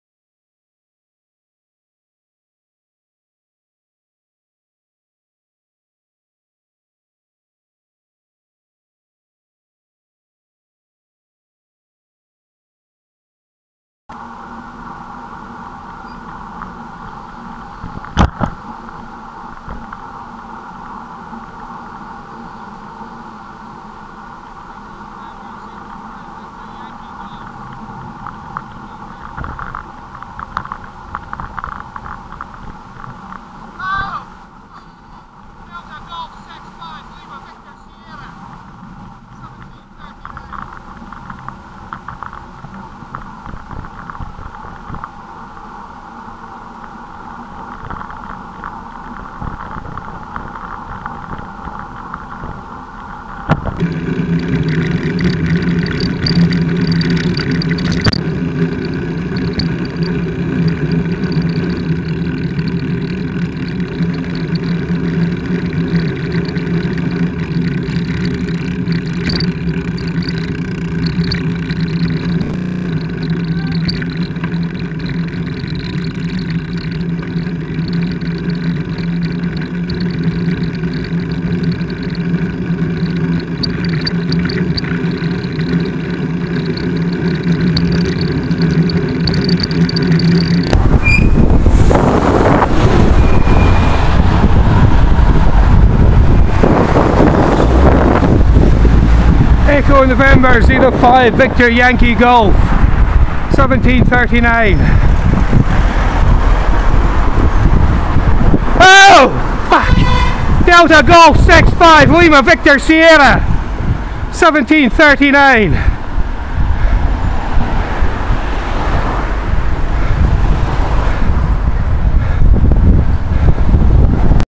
DG65LVS close pass finger
The driver can be seen giving me the finger at 0:37, if you can step through the video.